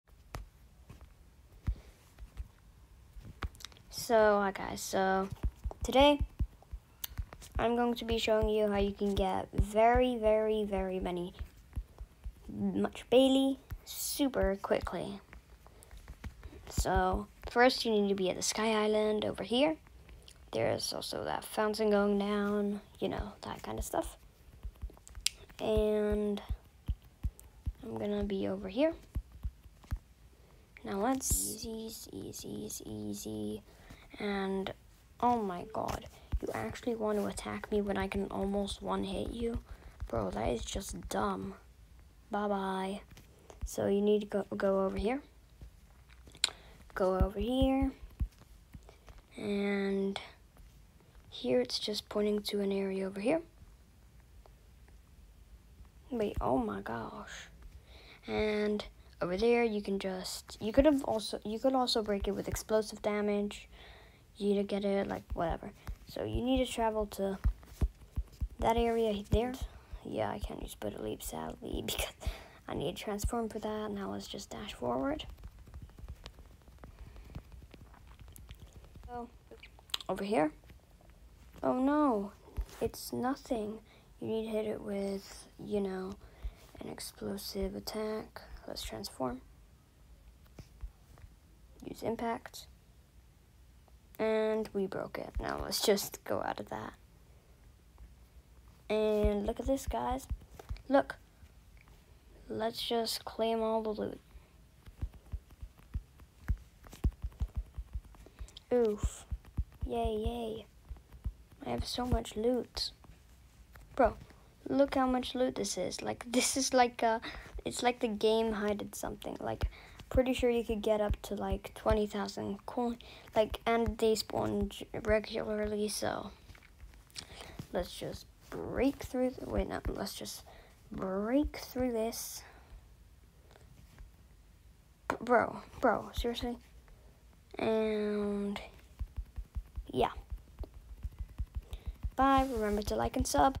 HOW TO GET MUCH BELI FAST!
Hello Watch this video to find put How to get 10K beli in just 1 minute